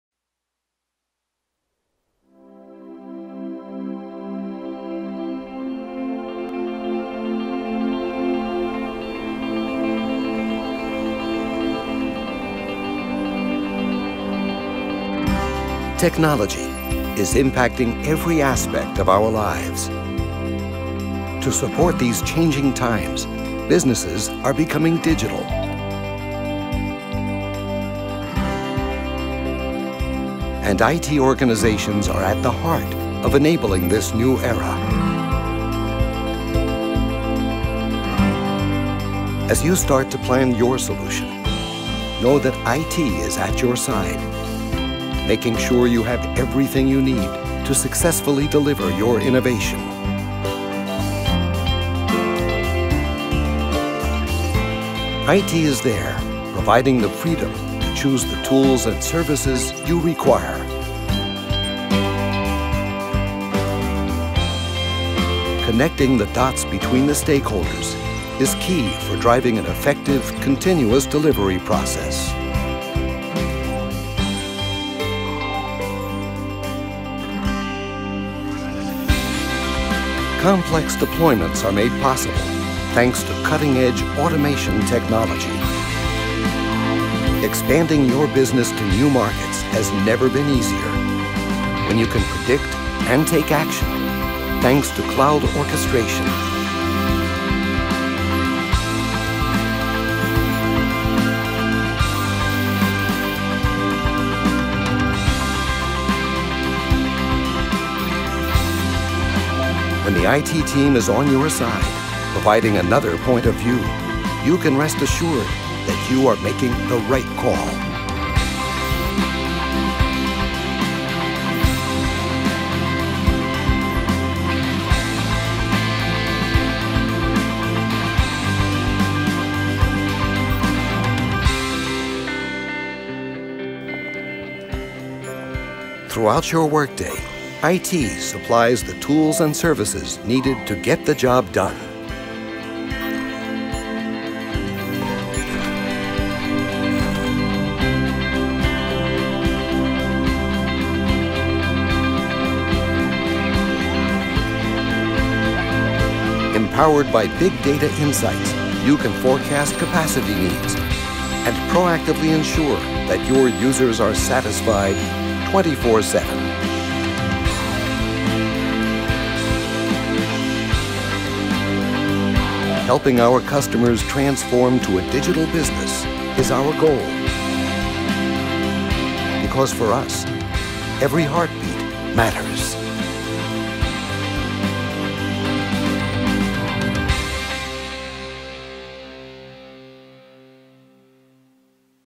HPE - IT Operations Management Vision
This vision concept shows how IT Operation management will look like in the near future.